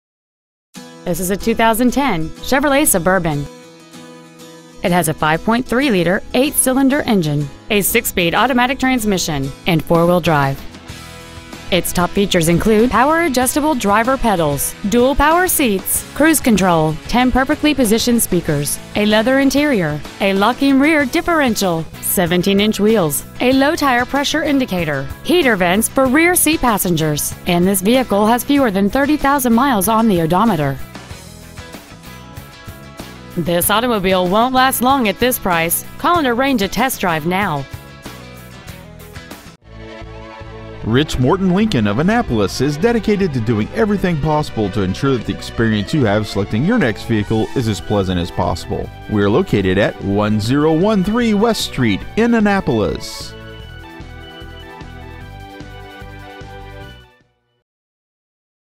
2010 CHEVROLET SUBURBAN MD
Year : 2010
 Make : CHEVROLET
 Model : SUBURBAN
 Engine : 5.3L V8
 Trans . : AUTO 6SPD
 Exterior : TAUPE GRAY METALLIC
 Miles : 29,009

 Stock : P229768

 Rich Morton Lincoln of Annapolis

 1013 West Street
  , MD 21401